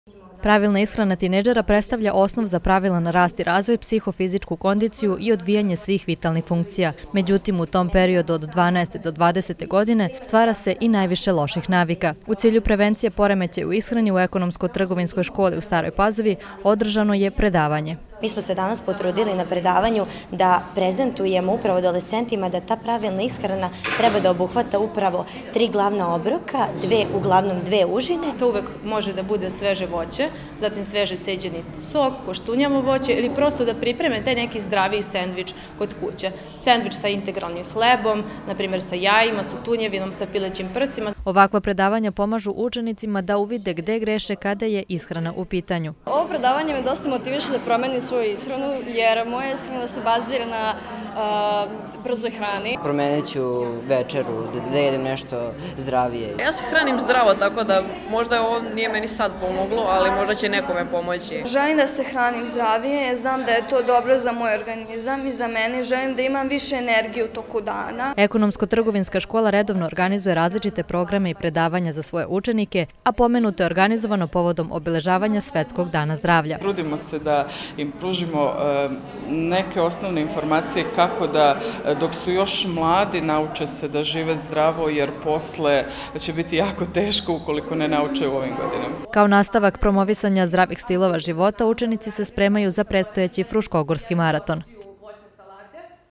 Predavanje o zdravlju u ETŠ
Predavanje o pravilnoj ishrani, istim povodom, imali su i srednjoškolci Ekonomsko - tehničke škole u Staroj Pazovi.